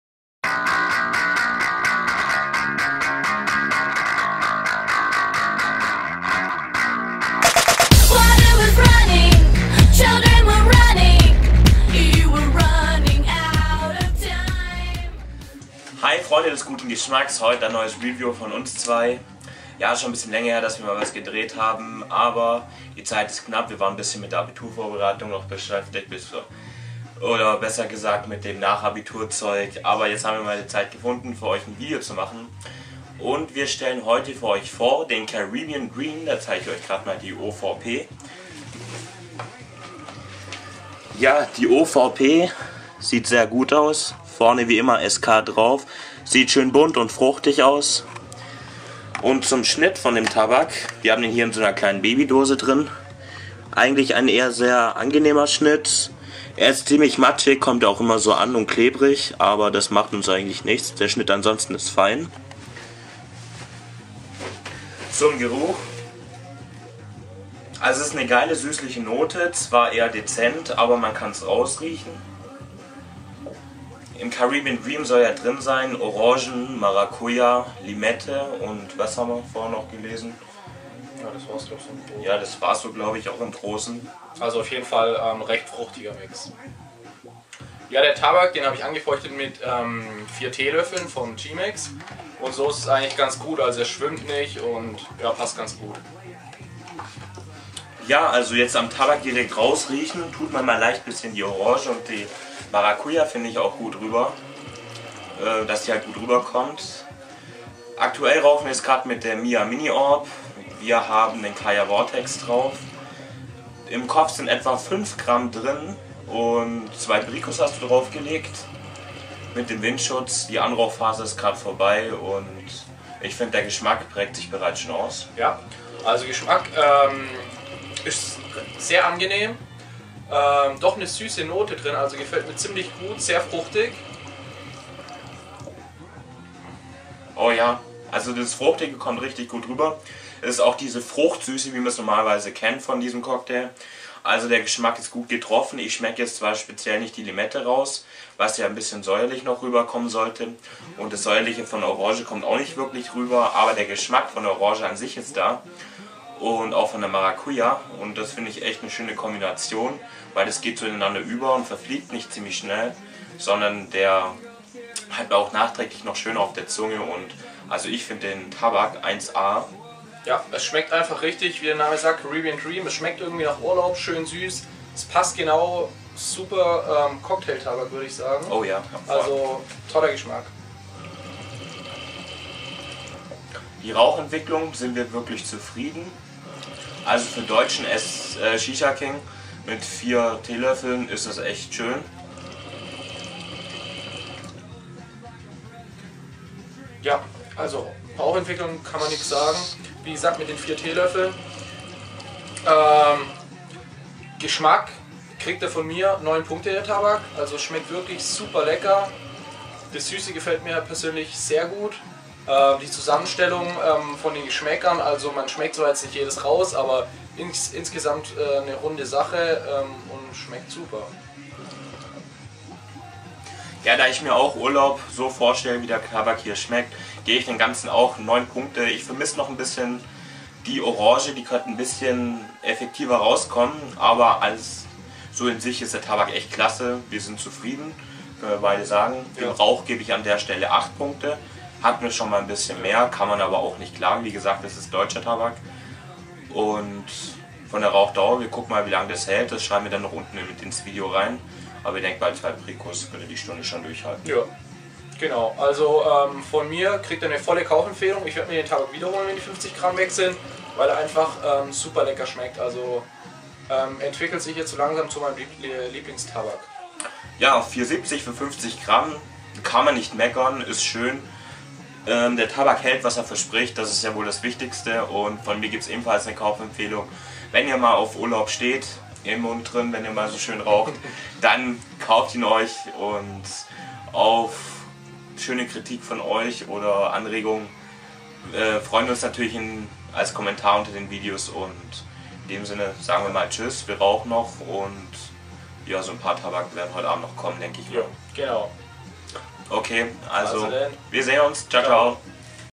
Shisha King - Caribbean Dream
Hey Leute, bei uns war heute mal wieder Testtag angesagt. Wir stellen euch unter anderem den Caribbean Dream von Shisha King vor.

Was wir erst allerdings später gemerkt haben ist, dass das ganze ein Wildberry Mangoi Flavour sein sollte, diesen Geschmack können wir nicht bestätigen, allerdings ähnelt er dem Cocktail sehr.
Orange und Maracuja waren eindeutig vom Geschmack her drin, auch wenn sie nicht auf der OVP aufgeführt sind.